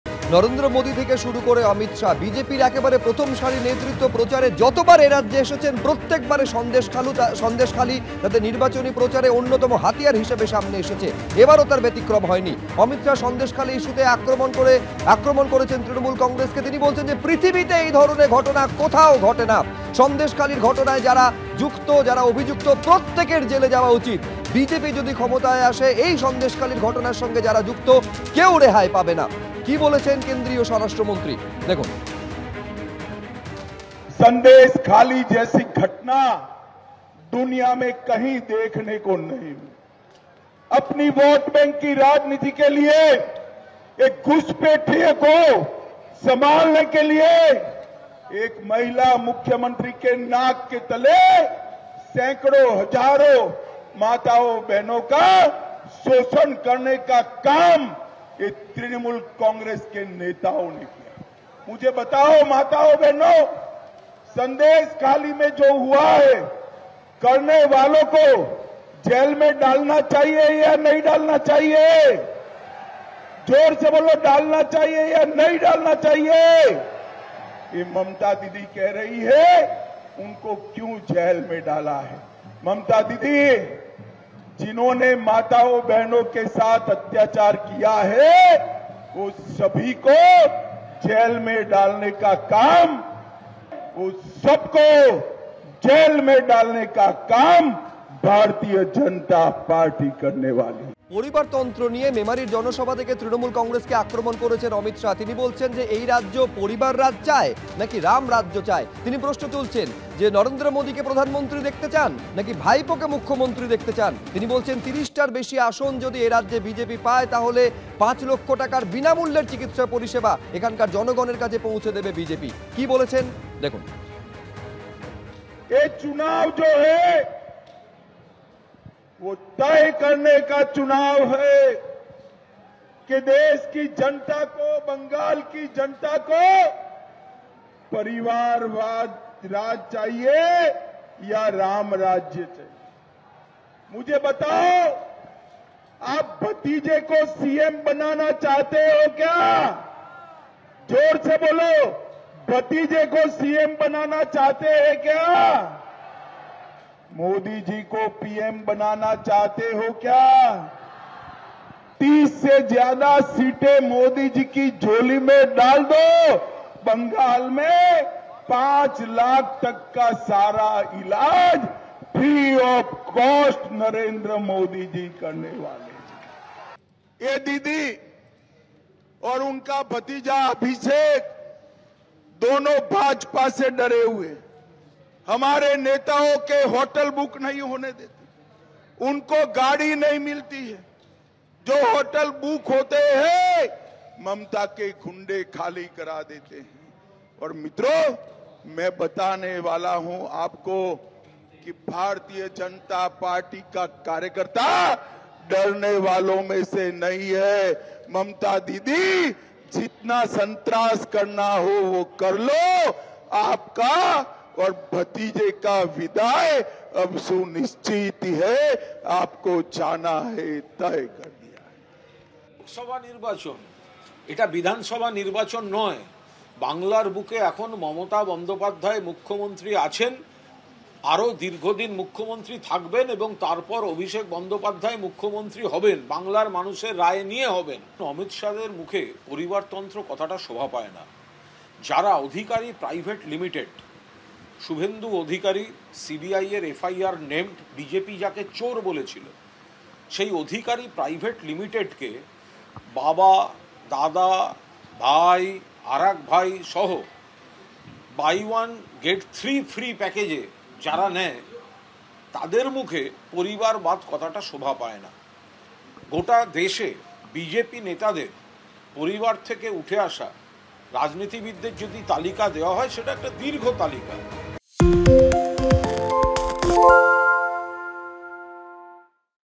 Amit Shah: 'সন্দেশখালির মতো ঘটনা পৃথিবীতে কোথাও হয়নি', আক্রমণ অমিত শাহর। ABP Ananda Live
West Bengal News: বর্ধমান পূর্বের বিজেপি প্রার্থী অসীম সরকারের সমর্থনে প্রচারে এসে ফের অমিত শাহের মুখে (Amit Shah) সন্দেশখালির (Sandeshkhali News) প্রসঙ্গ, 'সন্দেশখালির মতো ঘটনা পৃথিবীতে কোথাও হয়নি'। 'এর সঙ্গে যারা যুক্ত তাদের সবার জেলে যাওয়া উচিত', 'এখানে বিজেপির সরকার হলে এদের কেউ রেহাই পাবে না', বর্ধমানের মেমারির জনসভায় হুঙ্কার অমিত শাহের। ABP Ananda Live